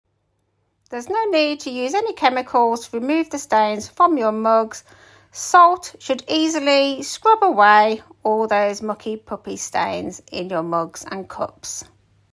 How to remove stains from cups and mugs.
This video demonstrates how to remove tea and coffee stains from cups and mugs using salt.